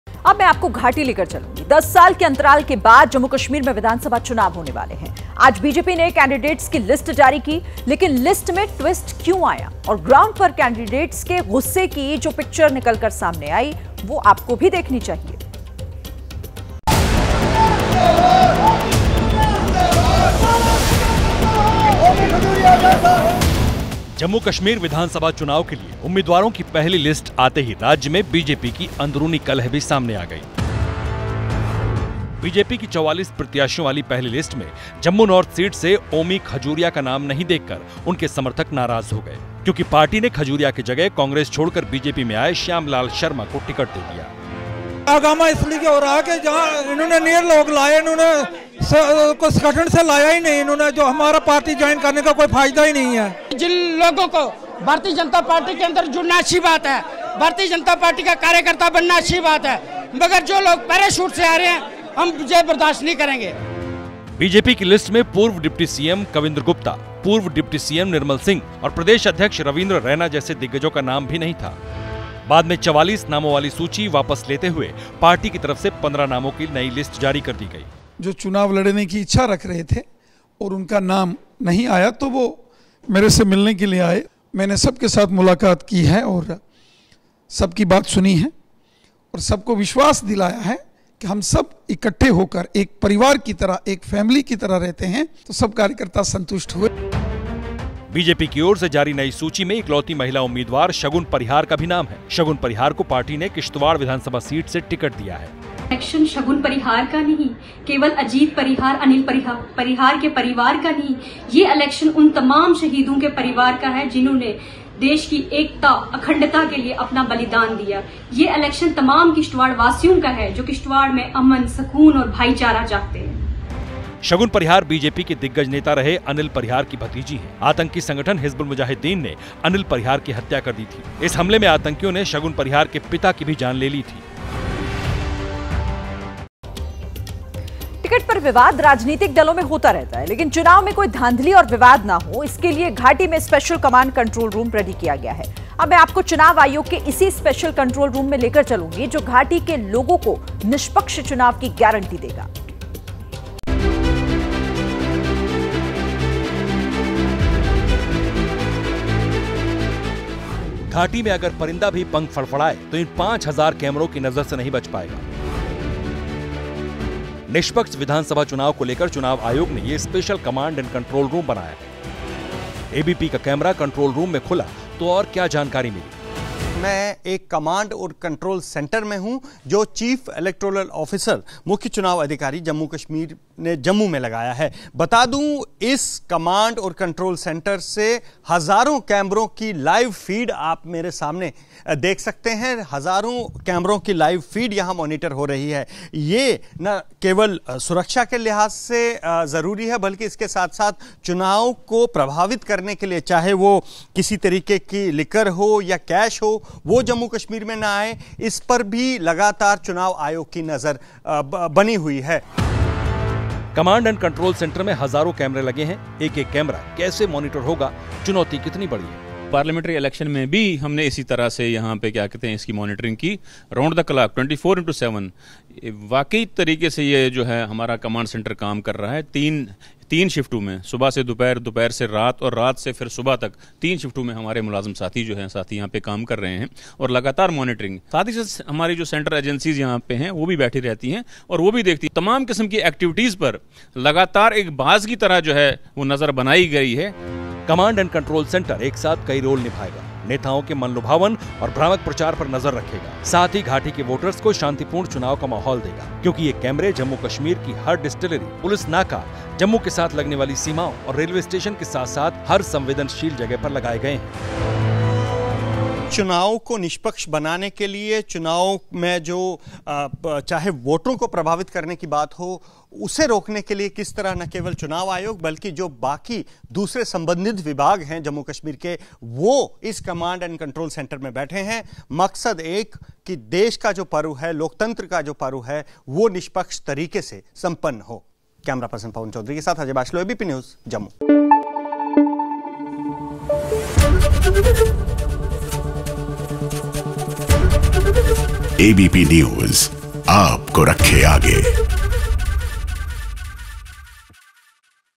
Jammu and Kashmir elections: BJP की कश्मीर लिस्ट में इतना ट्विस्ट क्यों है? Breaking News
10 साल के अंतराल के बाद जम्मू कश्मीर में विधानसभा चुनाव होने वाले हैं। आज बीजेपी ने कैंडिडेट्स की लिस्ट जारी की..लेकिन लिस्ट में ट्विस्ट क्यों आया? और ग्राउंड पर कैंडिडेट्स के गुस्से की जो पिक्चर दिखी,,.वो आपको भी देखनी चाहिए।